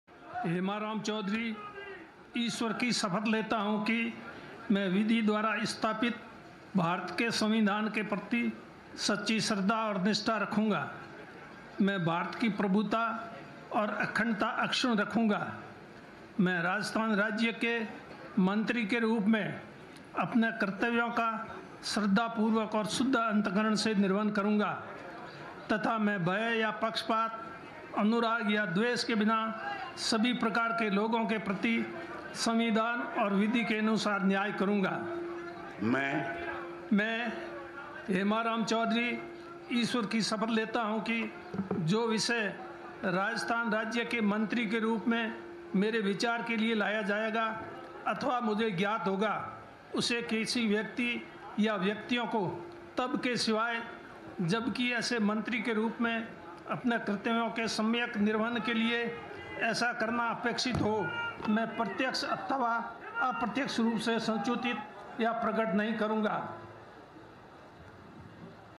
Hemaram Choudhary Takes Oath As A Minister In the Rajasthan Cabinet
RajasthanCabinetReshuffle | HemaramChoudhary takes oath as a Minister in the Rajasthan Cabinet.